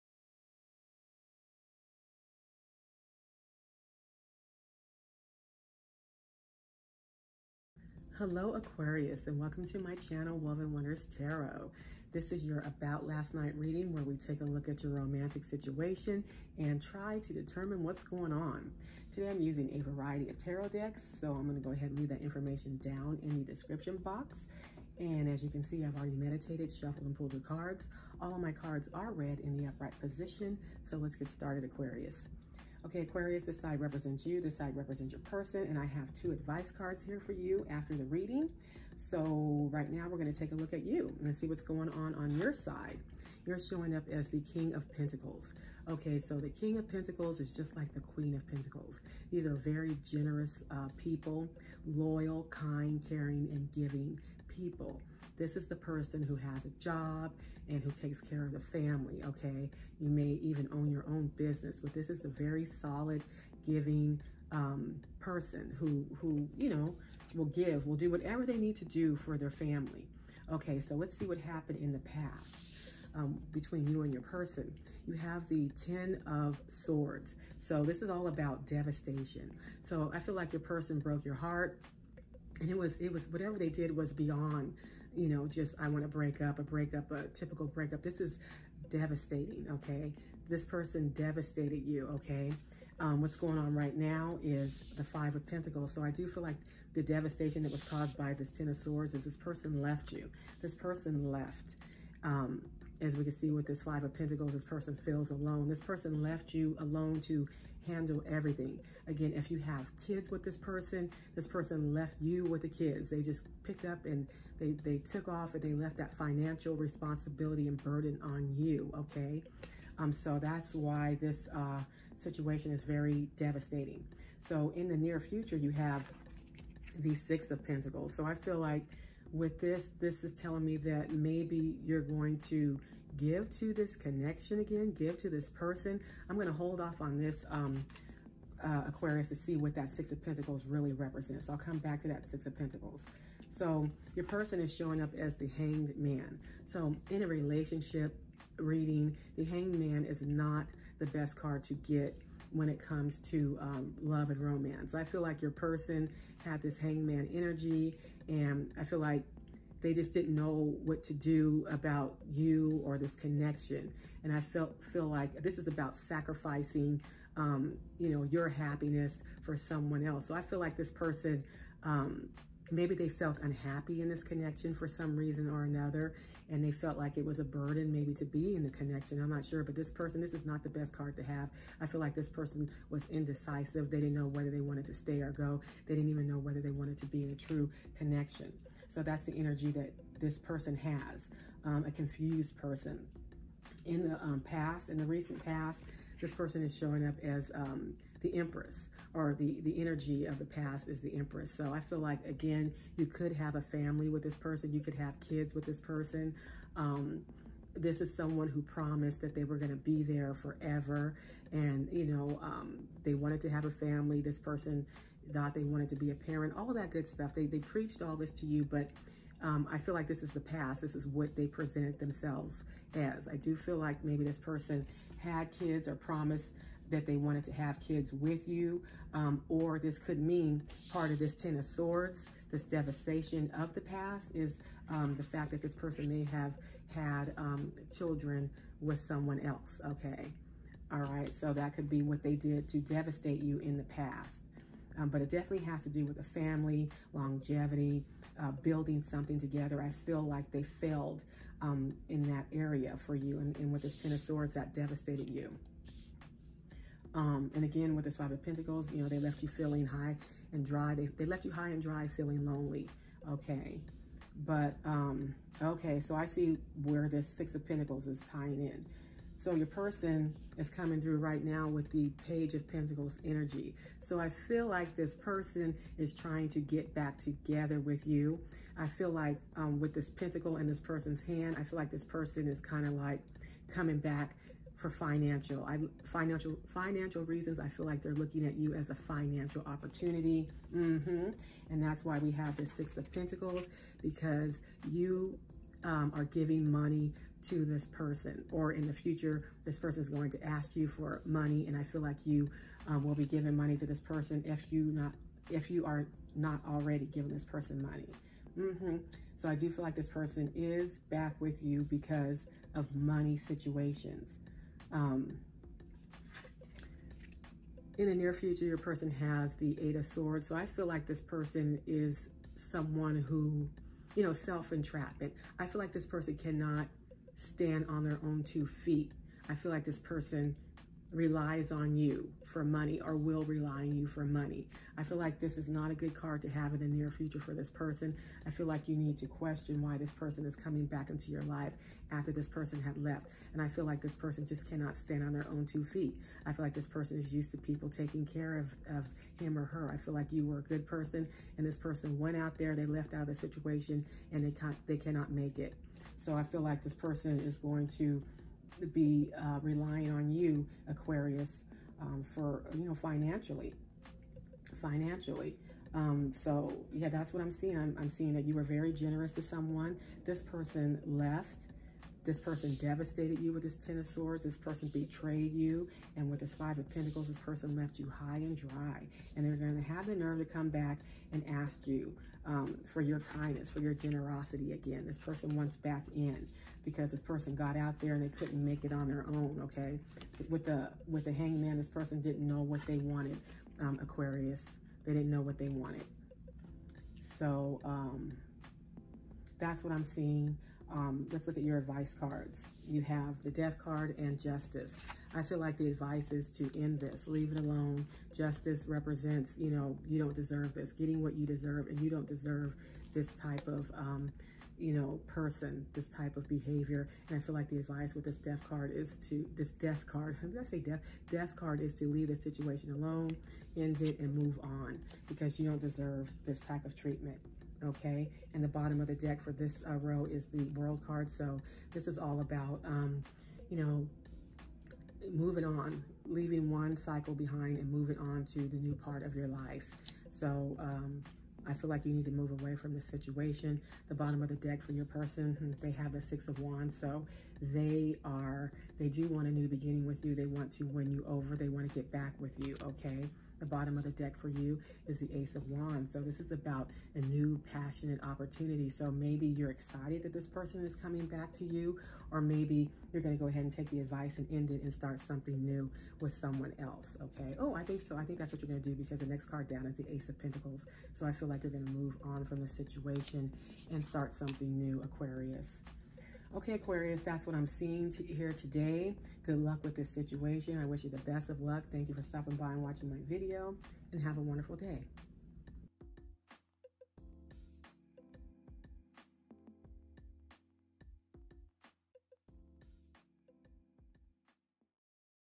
Aquarius ♒️💕This Person Betrayed You In The Past and Now They Want You Back!💋About Last Night!🎴
A Love & Romance Tarot Reading for Aquarius

Cards: The Light Seer’s Tarot: by Chris-Anne, The This Might Hurt Tarot Deck: Isabella Rotman, Double Major Arcana: Prime Muse – Published by Golden Age